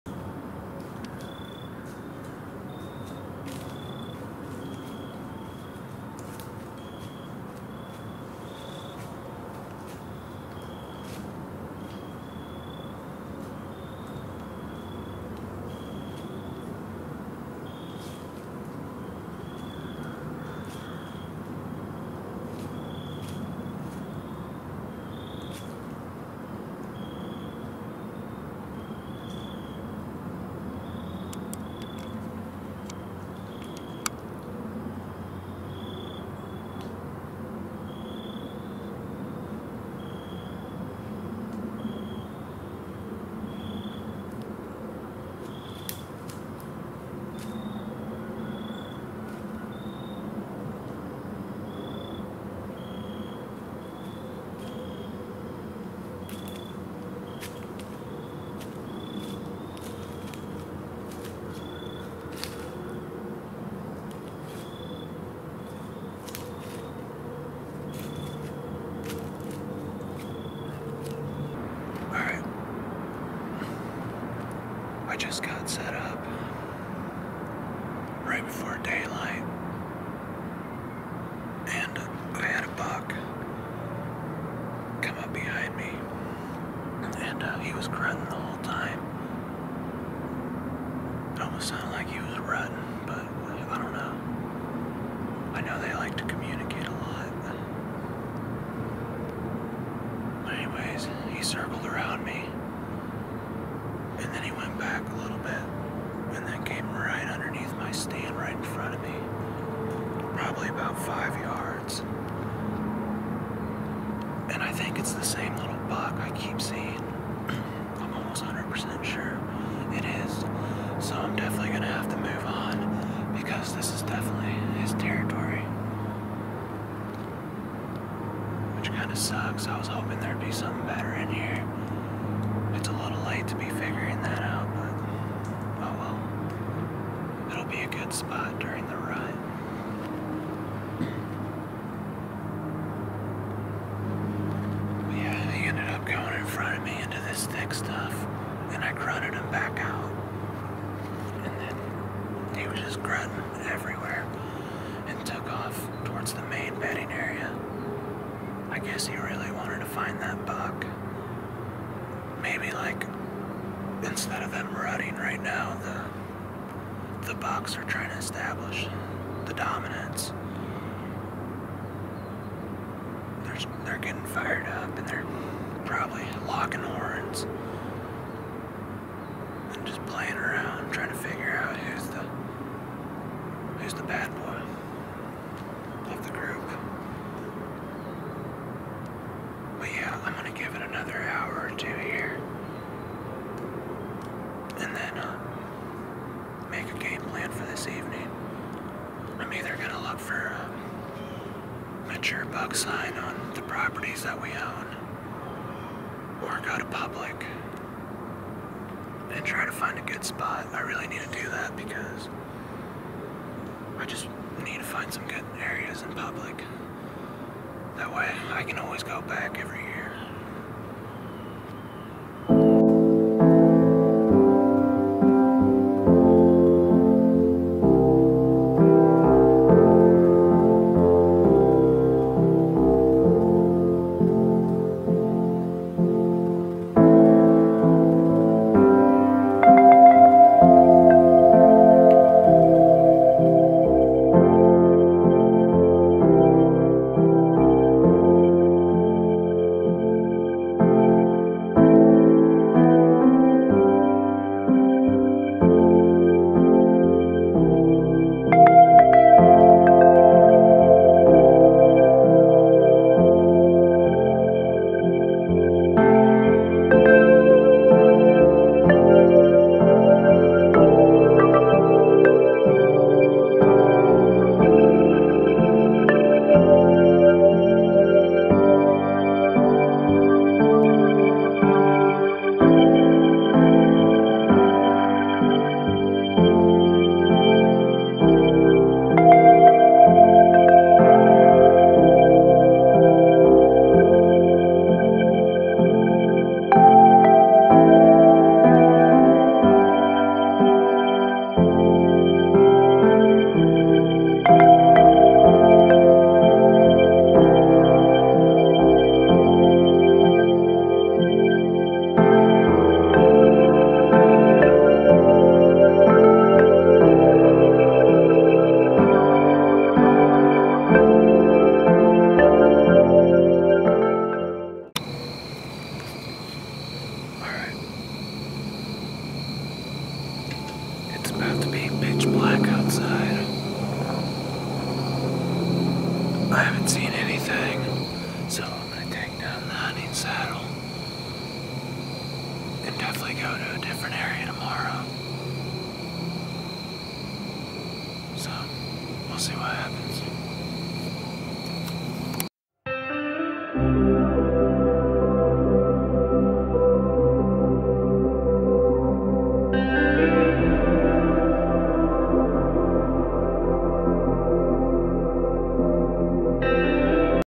DEER ARE STARTING TO MOVE IN MISSOURI!
In today's video I will be saddle hunting and scouting for a mature buck!